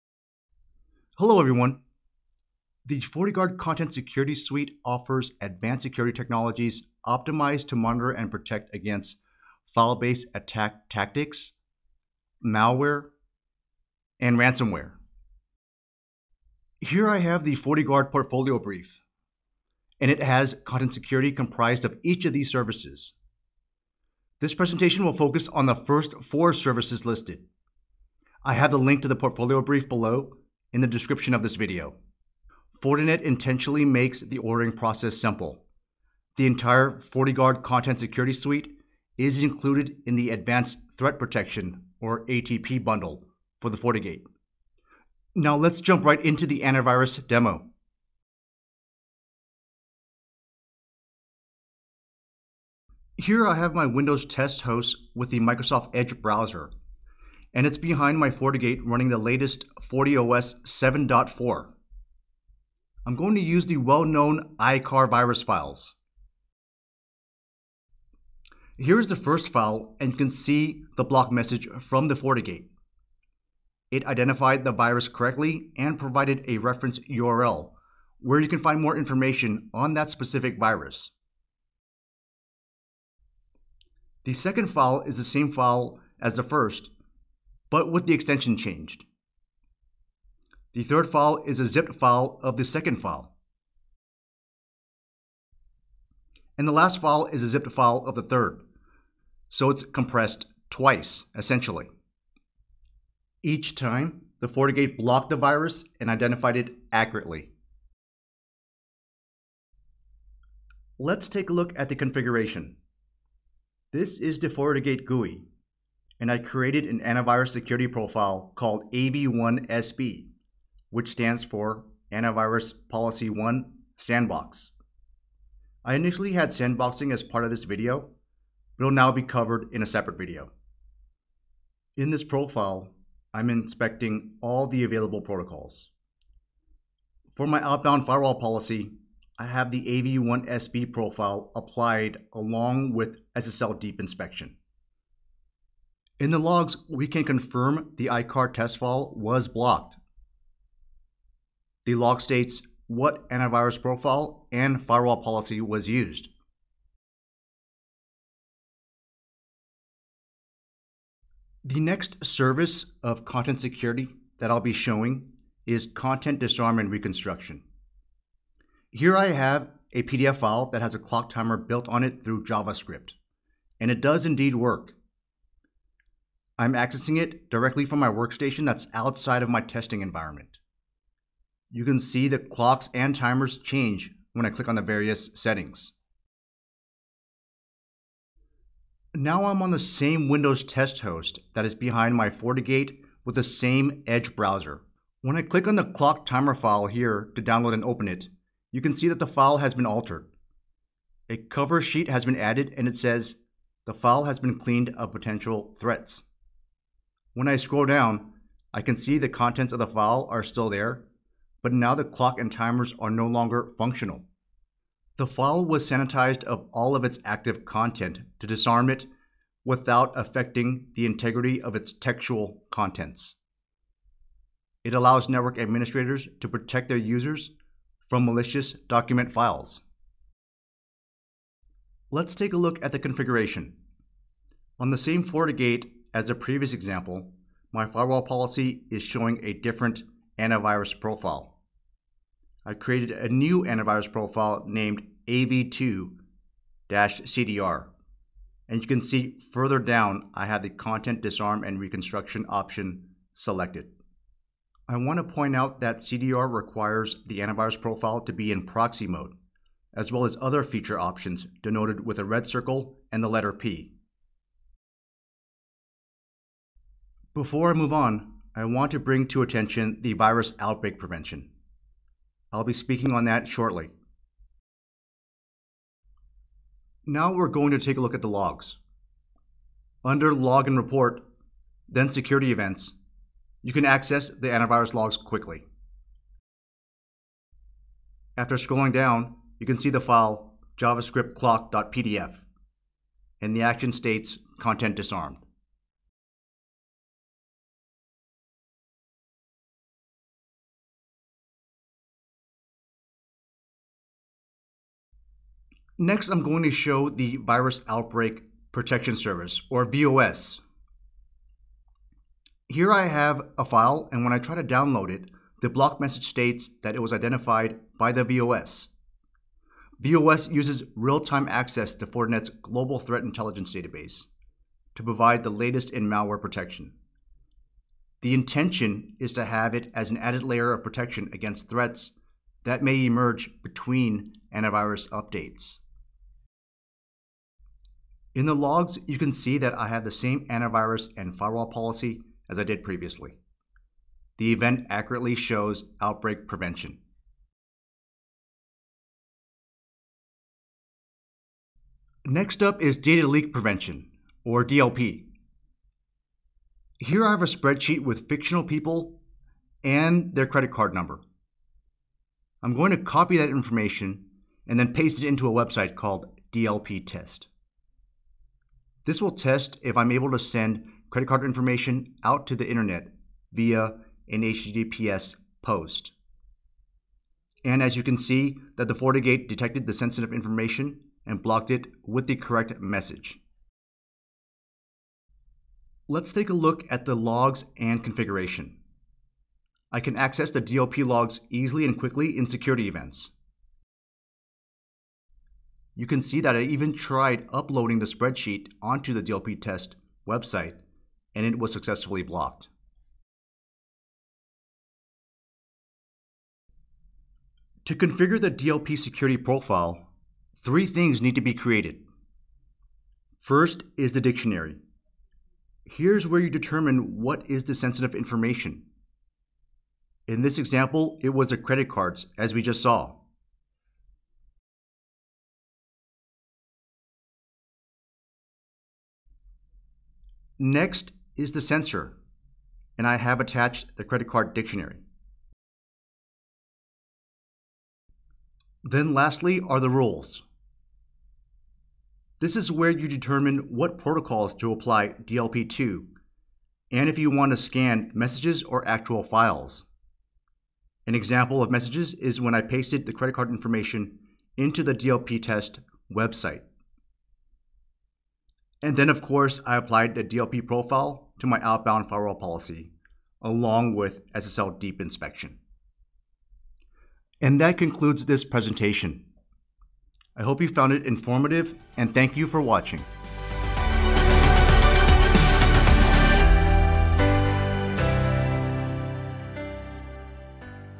FortiGuard AI-Driven Content Security | Product Demo
Fortinet's FortiGuard Content Security suite includes advanced technologies optimized to detect and protect against rapidly evolving targeted threats. These include ransomware, crypto-malware, and known and previously unknown file-based attack tactics.

Read the Fortinet blog to learn more about our business technology solutions